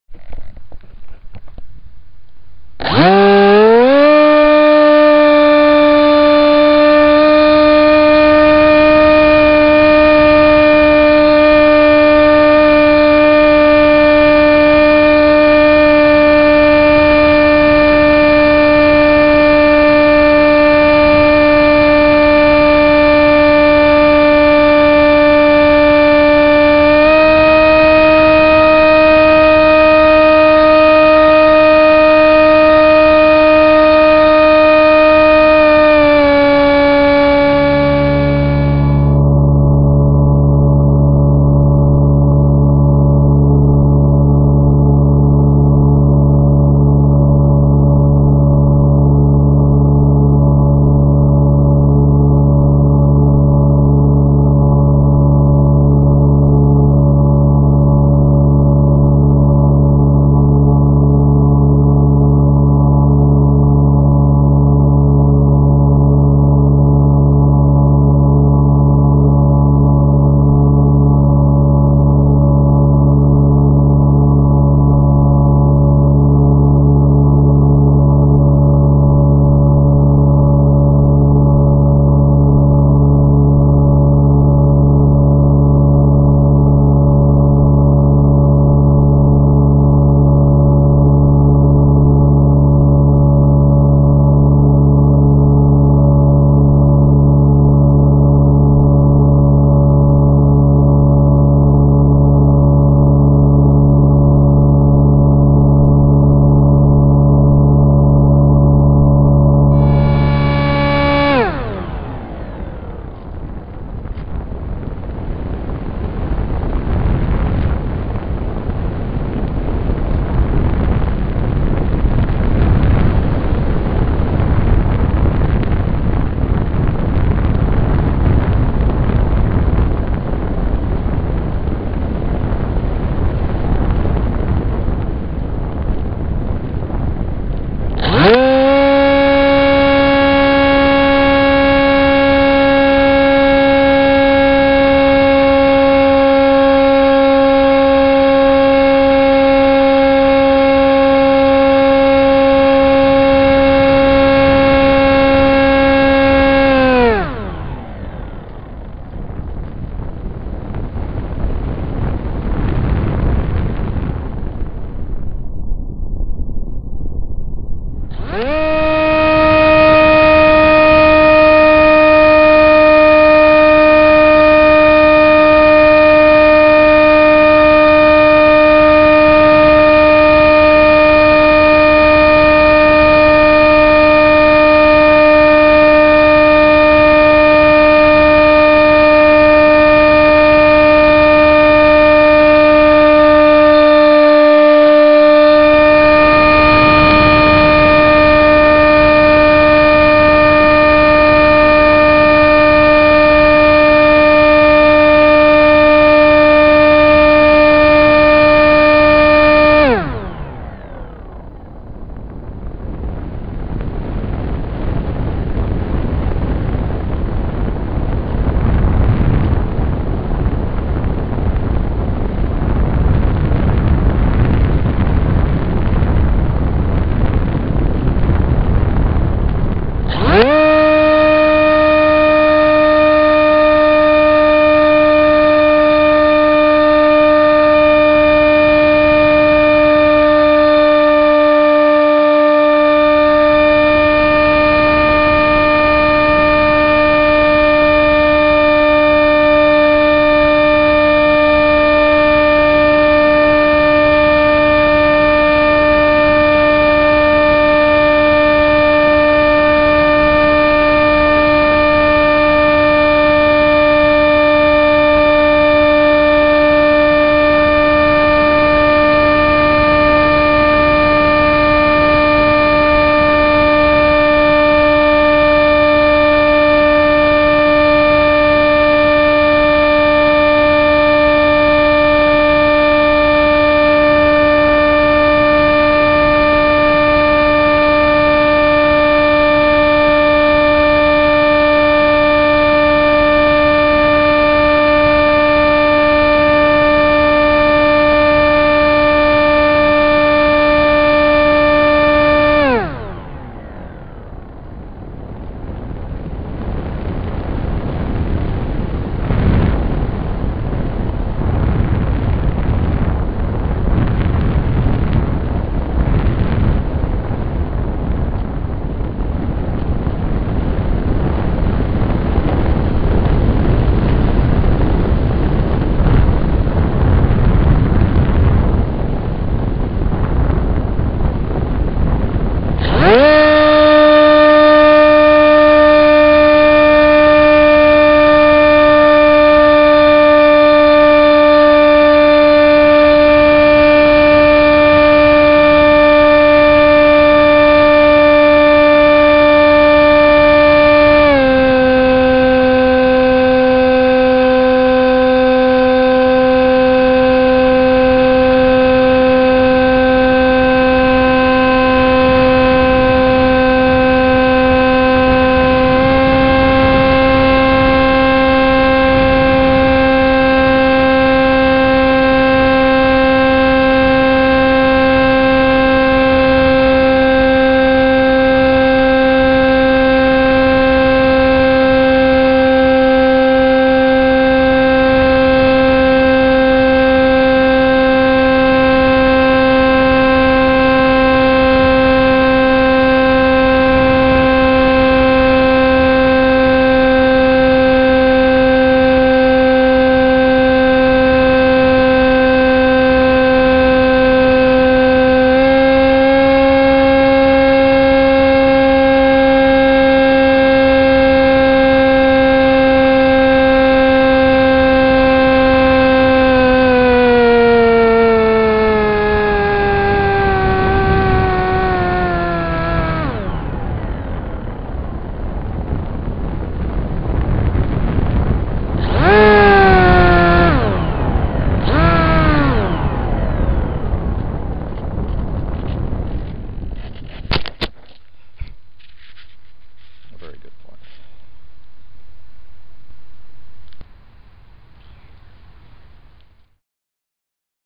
Pre Game Fly Bye
Took the MiG Delta with the Kodak DVR attached up for an evening spin before the Big High School Football Game between my Alma Mater TH North and their arch rivals TH South High School for the annual custody of the Liberty Bell. Got some aerial views of the stadium and gathering crowd.